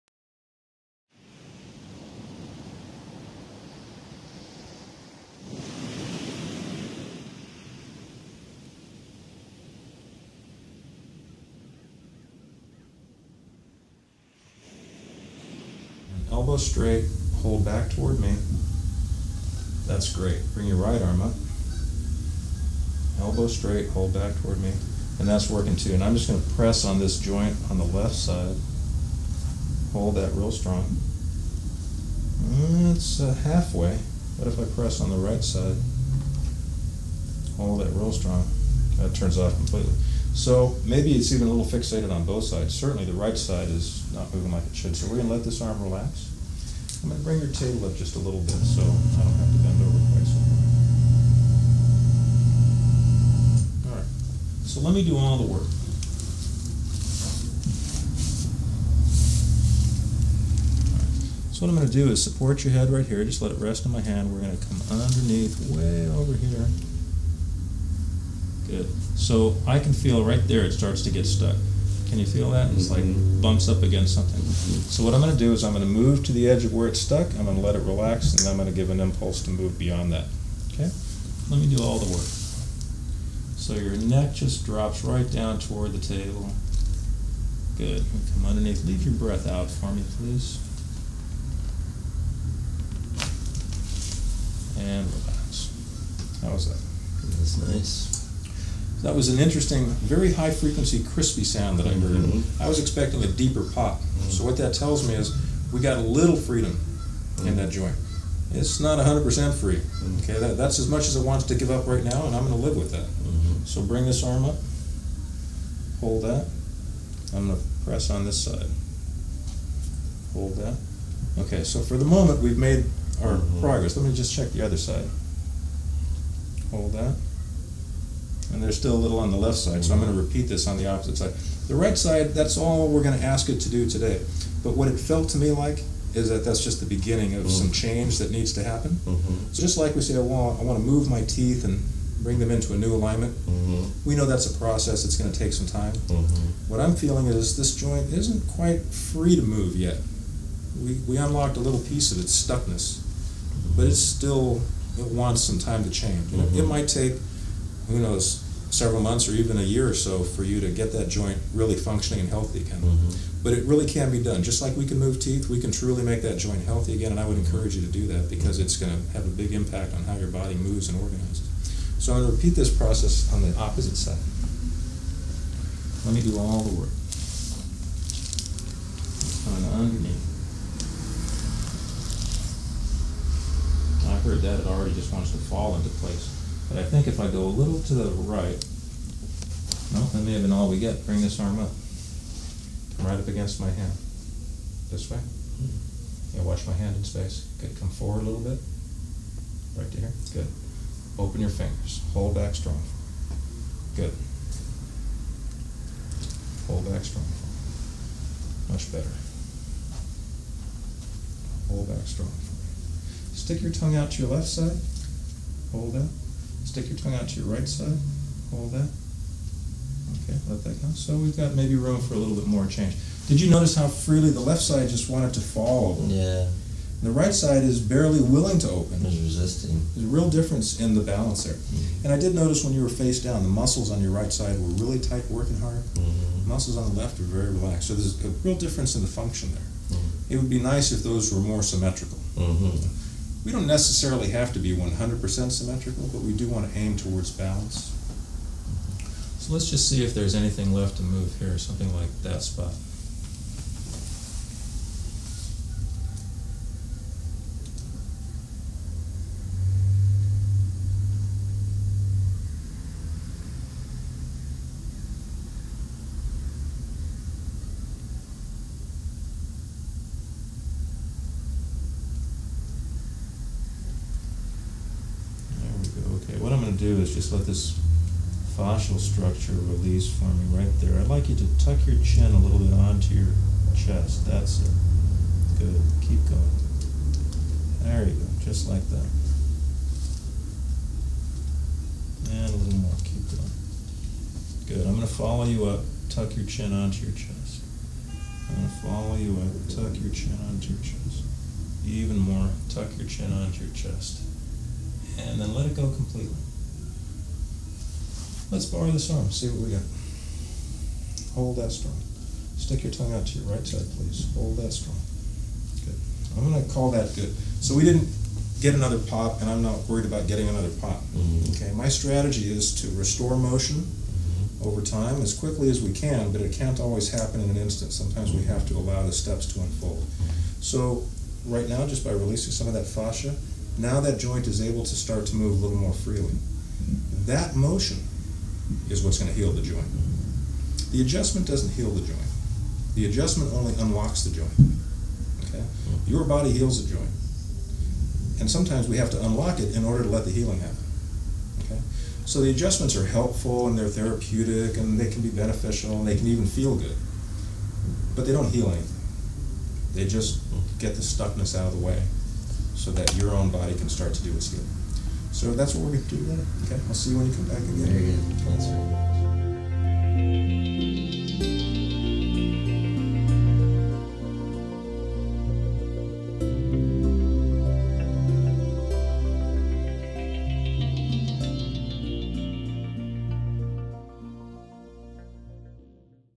Chiropractic Balance and Motion Dr. Stephen R. Gunter, D.C.
In this chiropractic video Dr. Stephen R. Gunter, D.C. does a manual adjustment for Tim to create balance and motion.

Video by Marlene Krueger and James P. Krueger